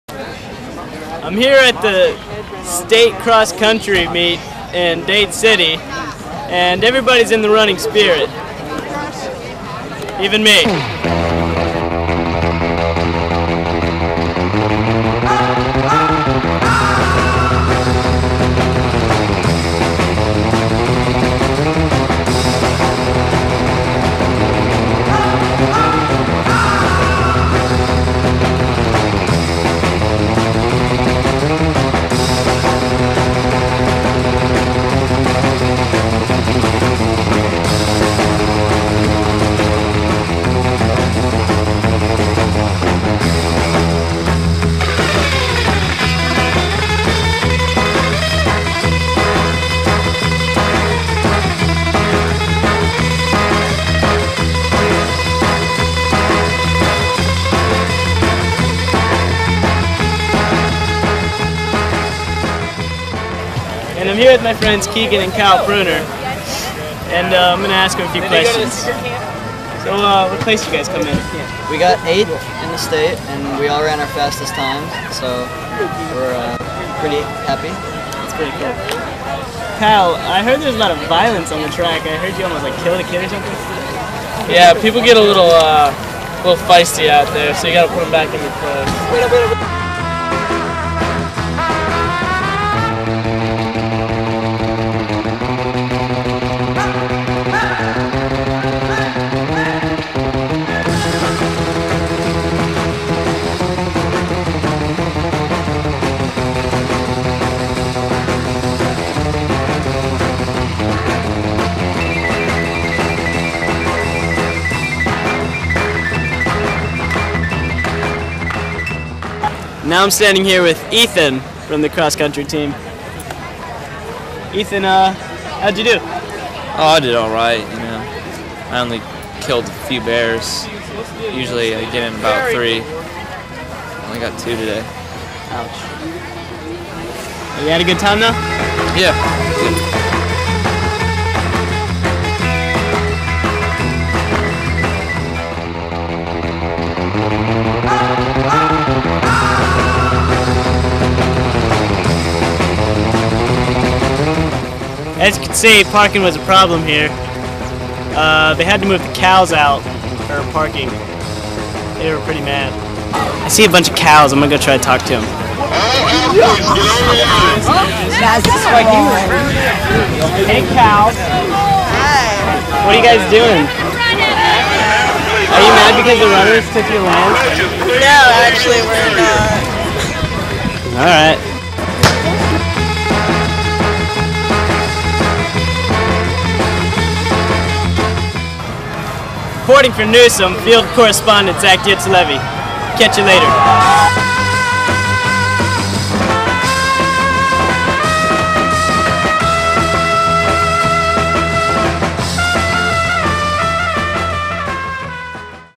Newsome XC State Meet
Zach does some interviews with the team and some footage of the race.